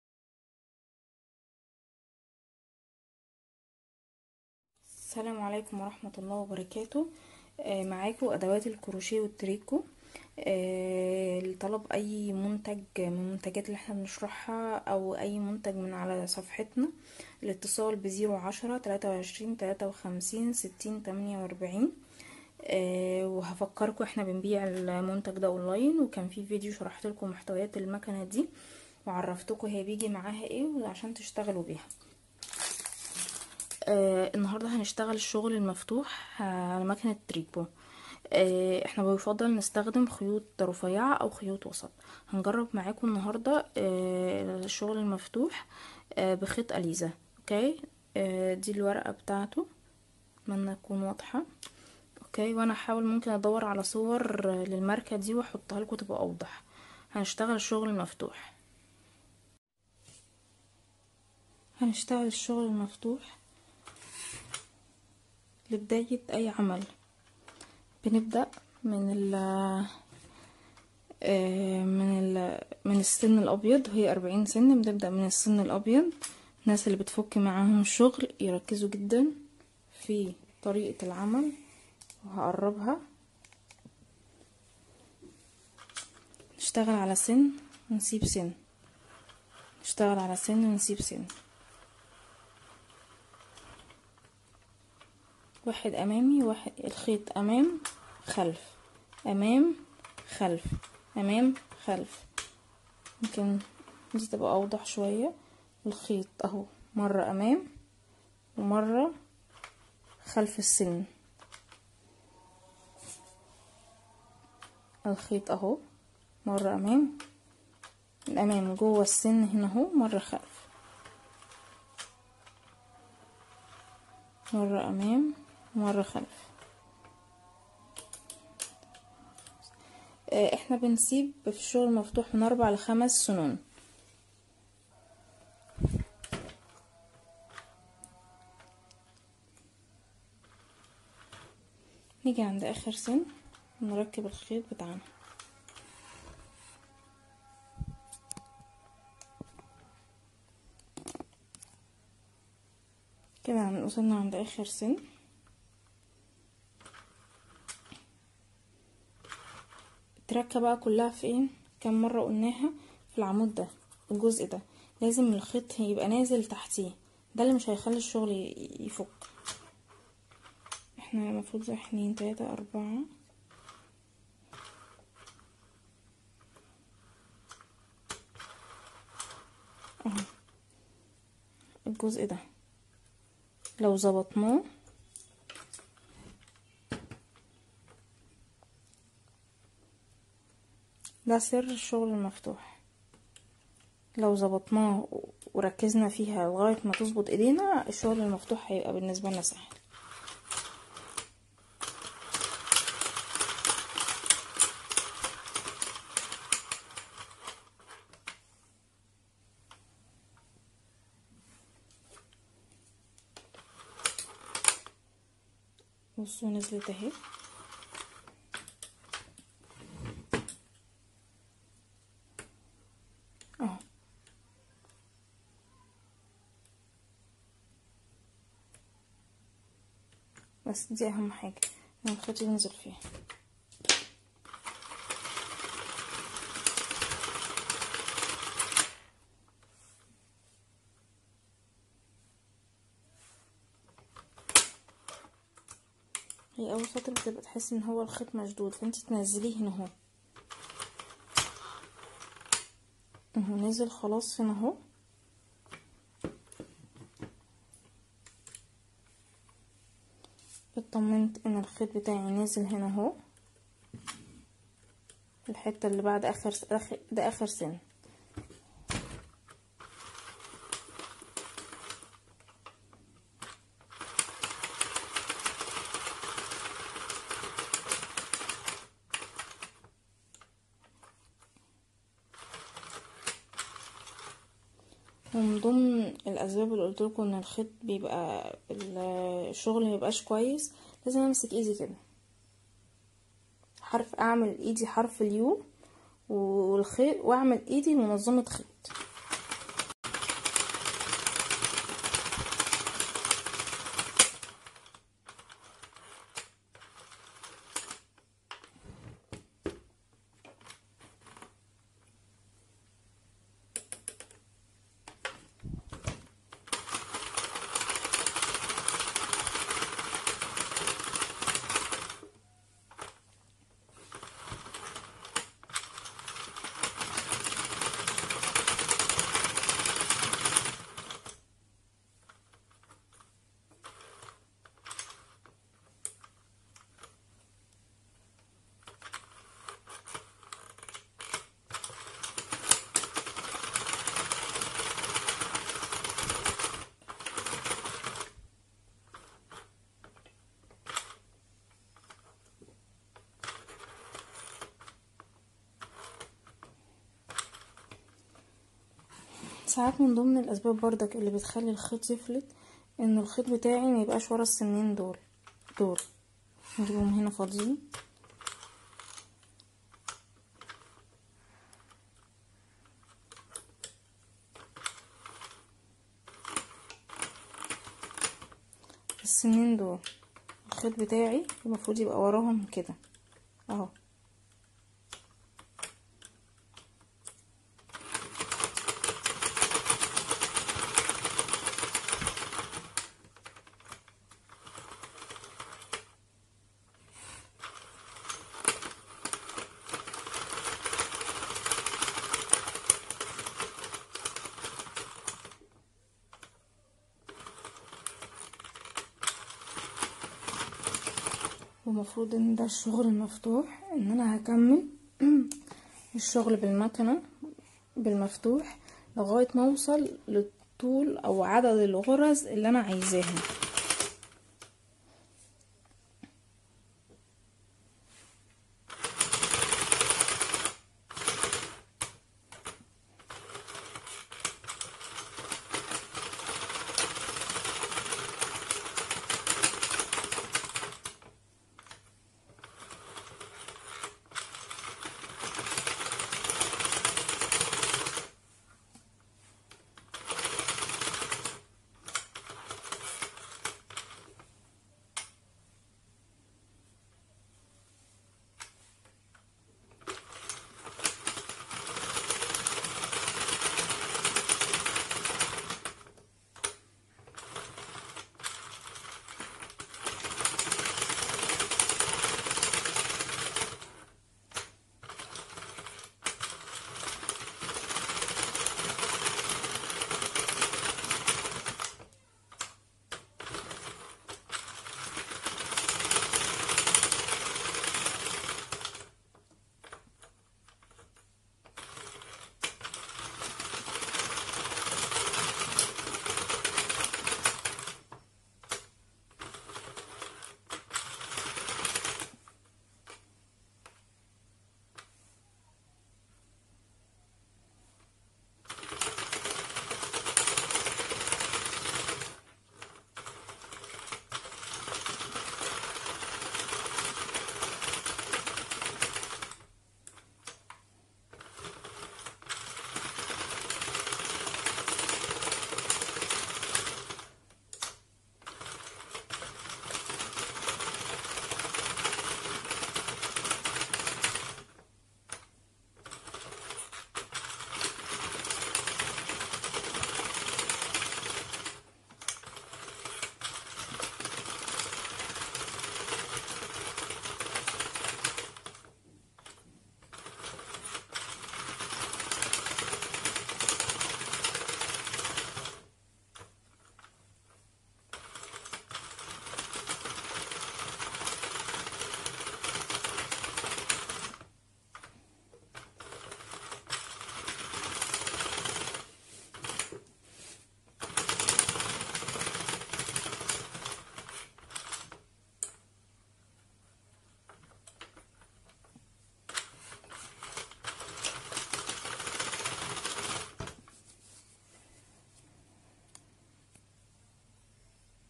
الشغل المفتوح للمكنة لعمل ملابس
مكنة التريكو 40سن
كلو بيسال بتعمل اية 👈👈👀👀👗👕ملابس اة ملابس 📢📣بلوفر فستان👗 جاكت 🧥بلوزه👕بوت👢 كاردن بس لا استني
🚴🚴 وبنطلون 👖بطانية اسكارف طاقية جيب 🤔🤔طيب ازاي وهي مدور 🎤🎤🎤هقول اشتغلي ع 35 سن رايح جاي هيطلعلك شرايح هنشبكة حسب اي مقاس عاوزة اعمل كذا شريحة 🤔🤔هتقول ممكن الشرح هقولك المكنة عندي مميزتها وهدايا 💝💝معها
1_اتنين سي دي يشرحلك بالتفصيل الشغل 📀💿
2_وقطع غيار 🛠️⚙️
3_ونول كور
☎️📞 لطلب المكنة التواصل عالخاص او الاتصال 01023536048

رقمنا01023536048